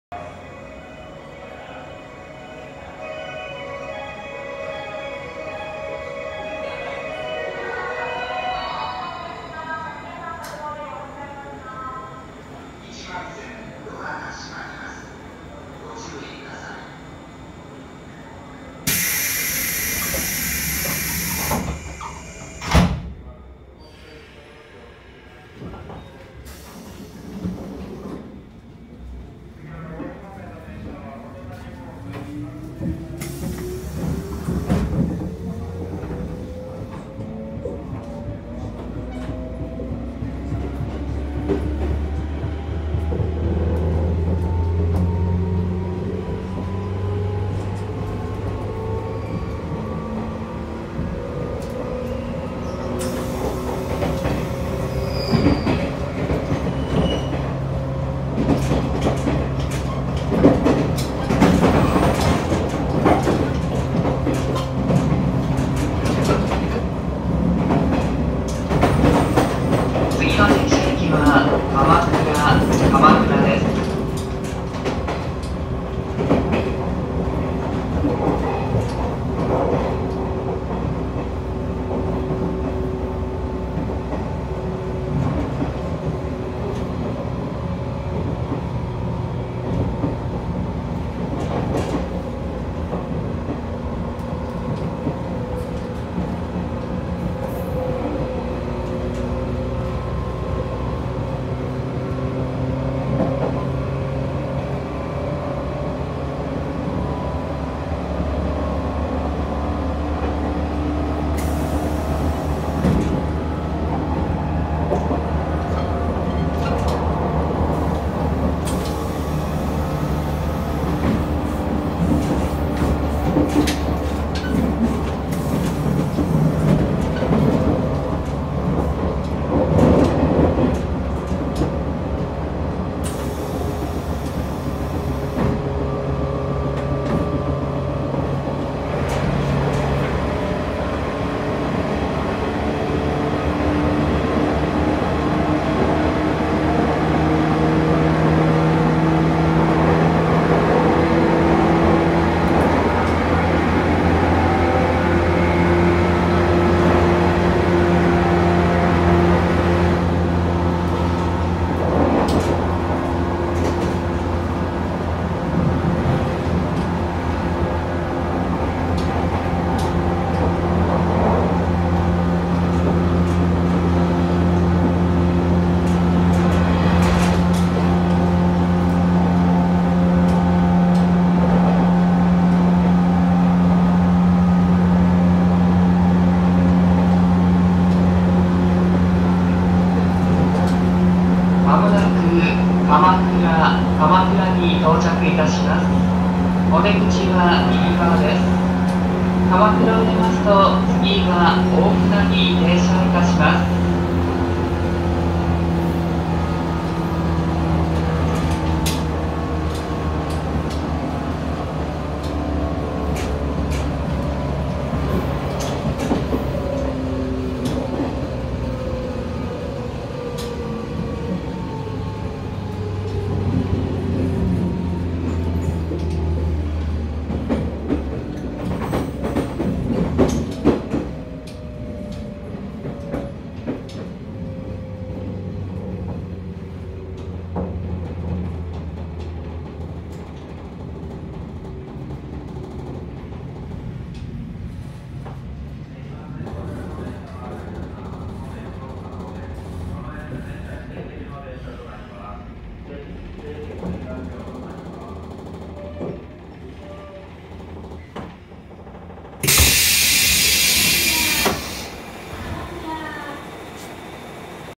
【185系走行音】横須賀線逗子→鎌倉走行音・車窓/185 series Zushi-Kamakura
横須賀線逗子〜鎌倉間の走行音・車窓です。
編成   185系B6編成
区間   逗子→鎌倉
種別・行き先   快速成田山初詣横須賀号成田行き